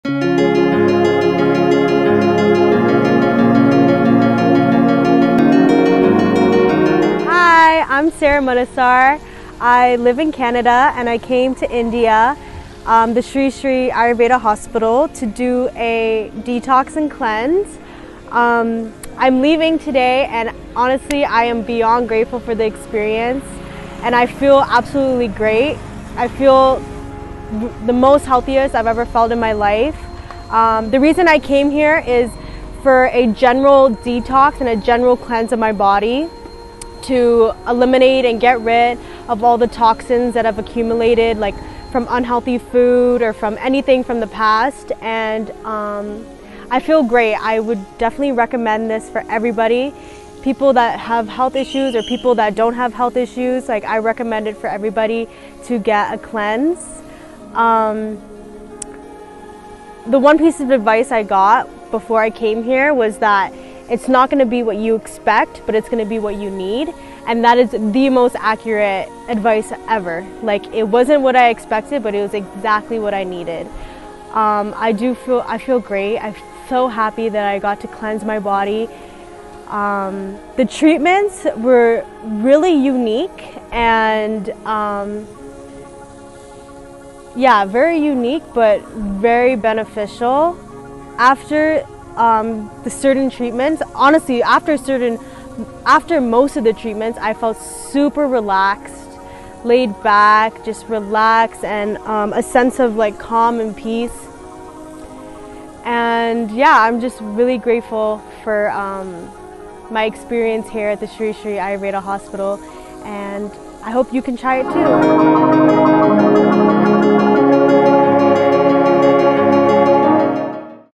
Detox and Rejuvenation with the Panchakarma Treatments at @srisriayurvedahospital Bangalore.
In this video, Sarah Manesar from Canada, shares her experience with us about the goodness of the Panchakarma Treatment that she underwent in our hospital during her stay. She was recommended with various types of Panchakarma treatment for Detox and cleansing of the body. She expressed that she is happy and feeling great after the Panchakarma Treatment. Check out her relaxing transformation tale with the goodness of Panchakarma Treatments at Sri Sri Ayurveda Hospital.

To learn about 3 ways to take care of digestive health,